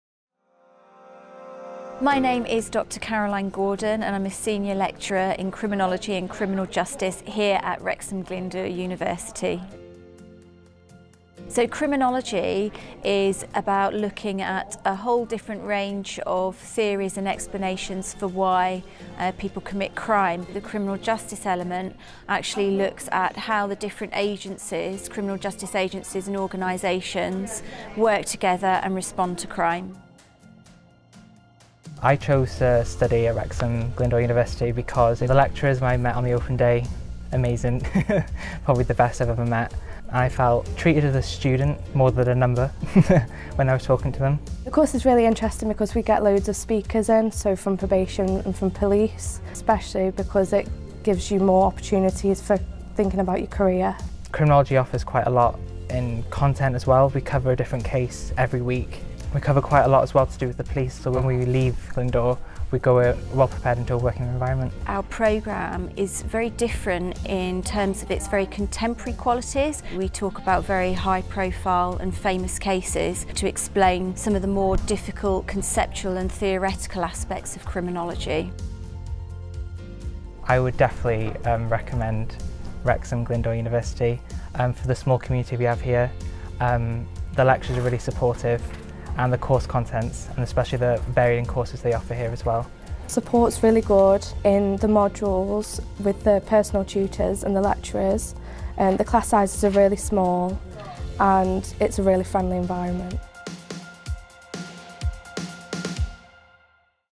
BA (Hons) Criminology & Criminal Justice - Hear from students on the course at Wrexham Glyndwr Uni
Thinking of a career in Criminology or Criminal Justice? Hear all about our course from Dr Caroline Gorden, Senior lecturer in Criminology & Criminal Justice along with students on on the course at Wrexham Glyndwr University.

You are currently visiting the official YouTube channel of Wrexham Glyndwr University, a higher education institution based in north Wales, UK.